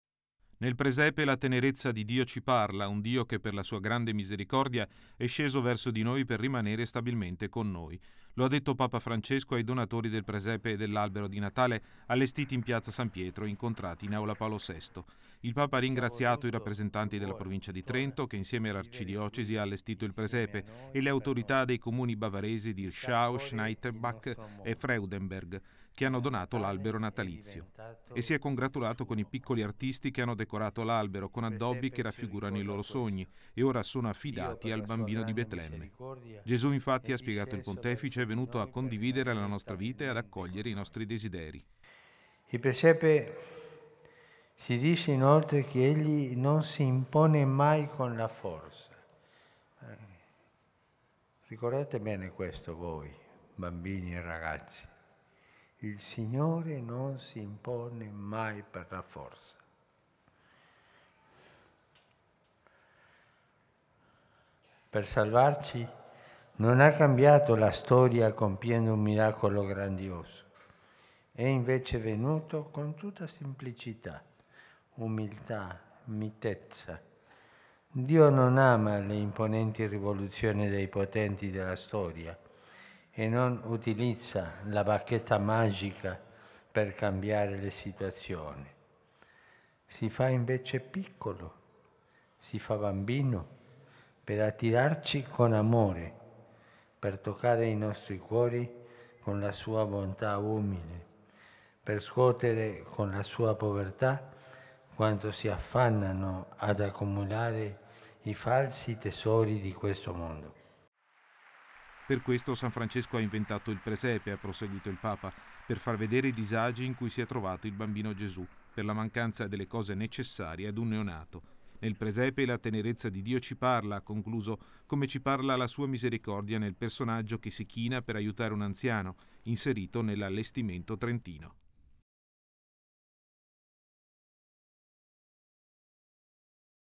Udienza donatori presepe e albero piazza San Pietro 18-12-2015
Nel presepe ci parla la tenerezza di Dio

Papa Francesco ha incontrato in Aula Paolo VI, i donatori del presepe e dell’albero di Natale allestiti in piazza San Pietro.